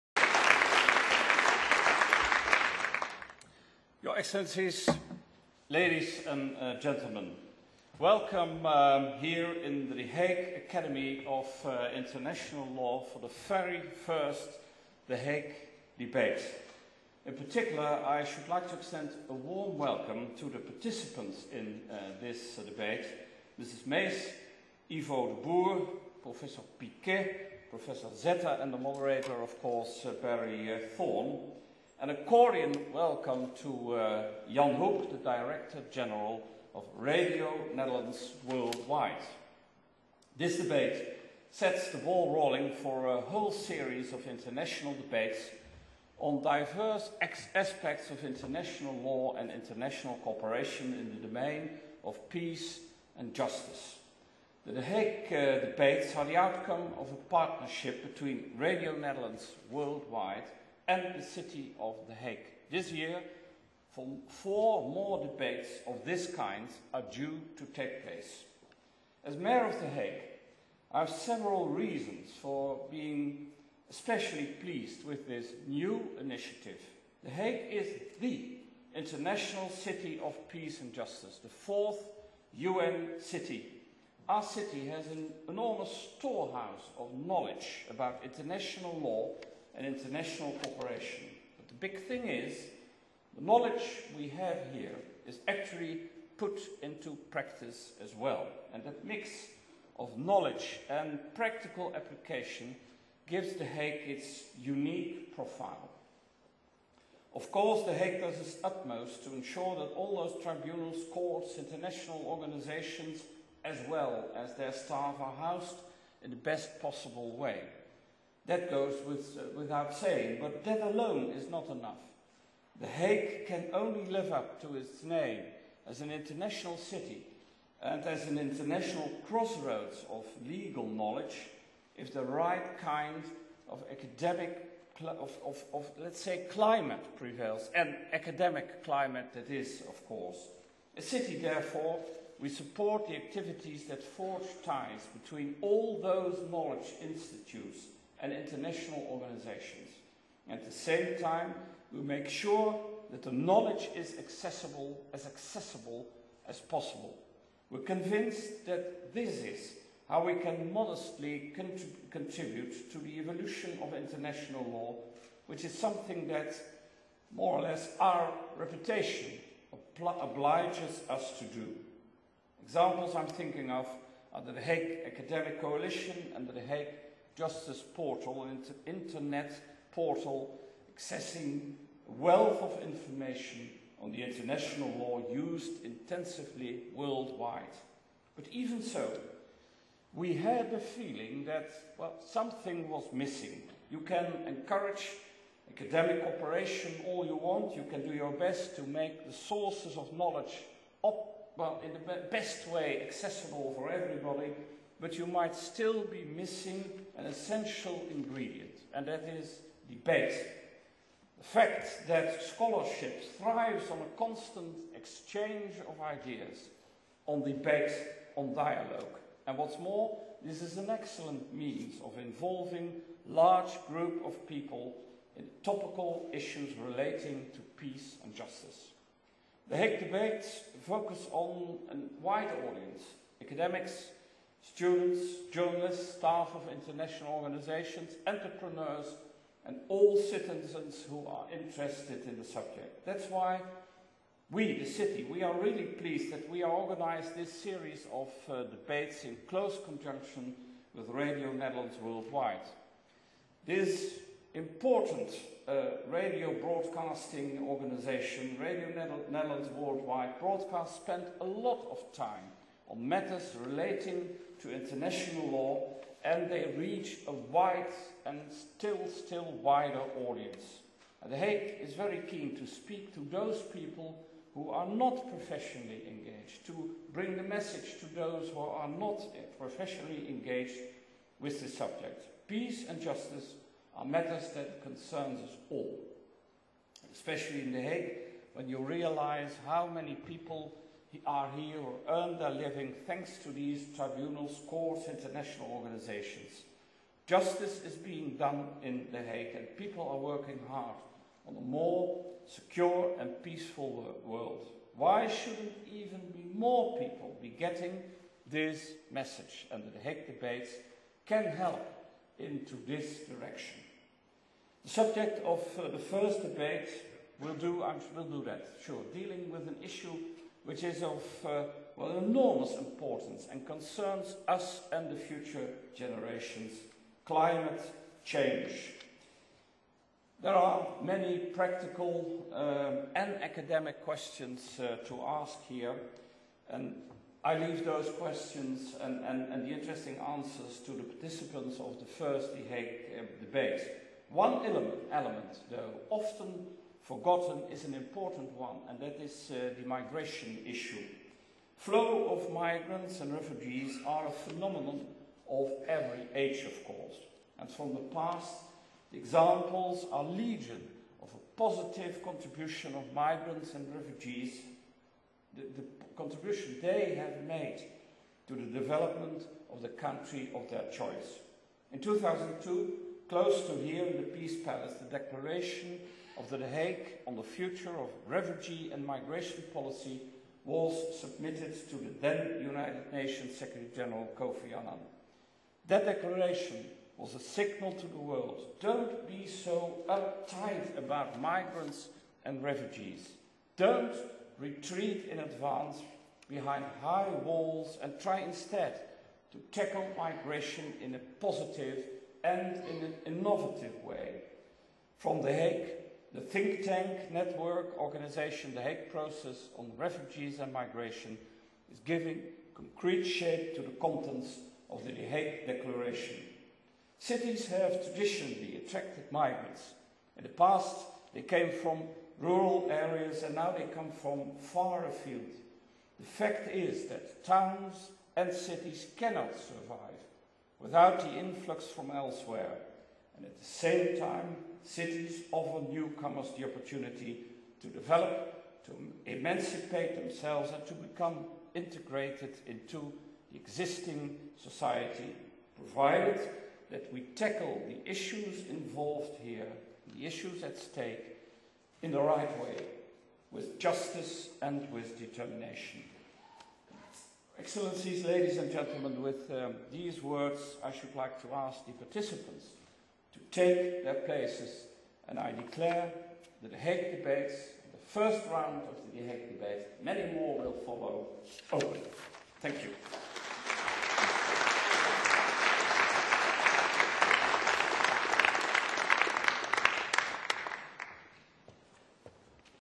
Opening of 'The Hague Debates' by Mayor Jozias van Aartsen
The City of The Hague and Radio Netherlands Worldwide are organising a series of debates about international peace and justice.